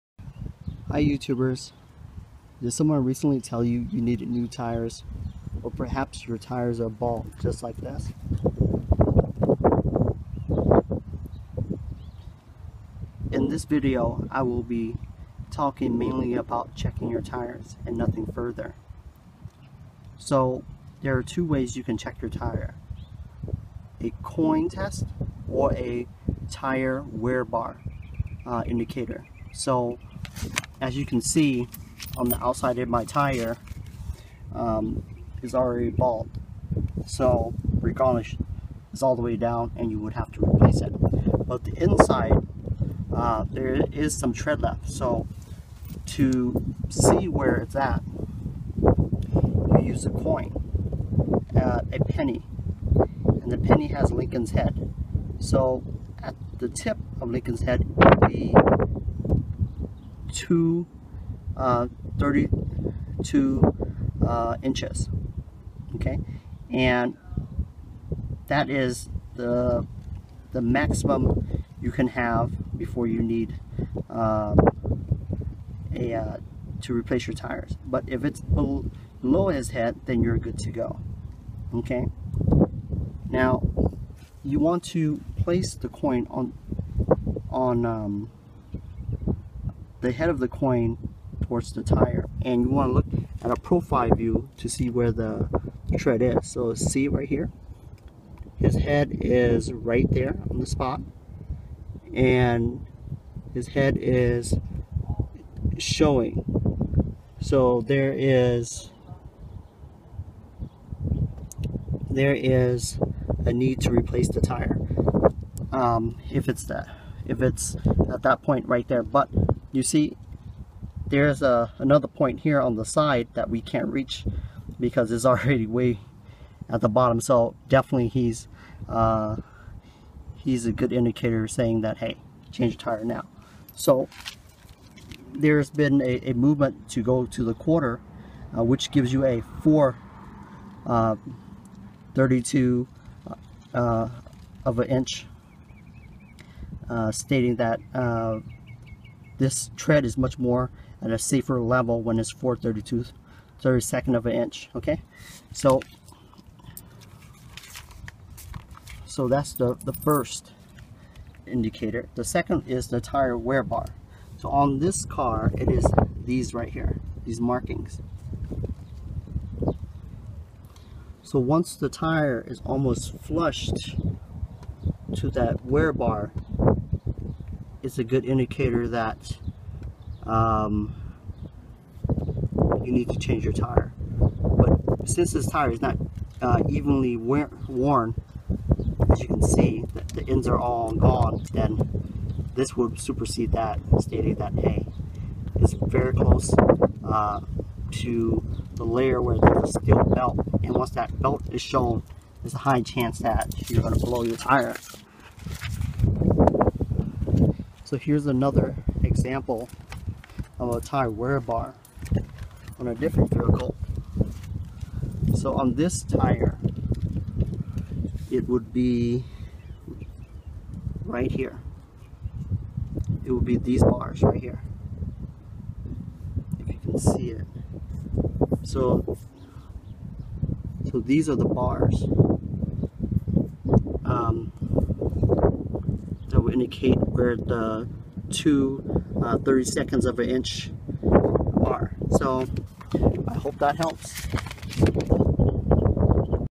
Do you need new tires? 2 Simple Ways to Check
How to check tires for wear.